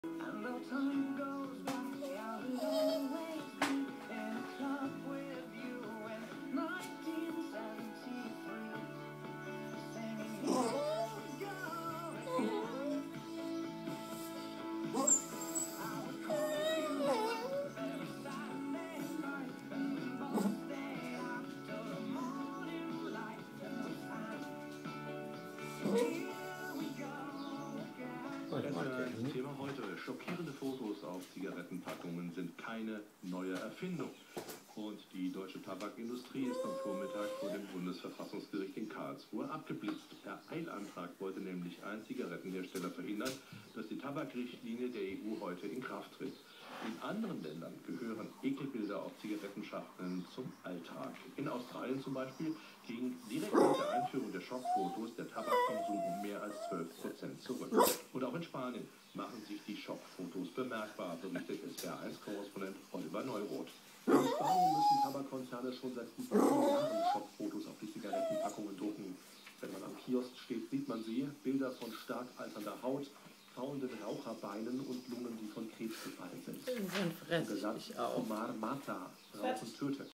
Boston terrier- "gleich sagt er was"
Boston terrier- "gleich sagt er was", sitzt am Tisch und meckert...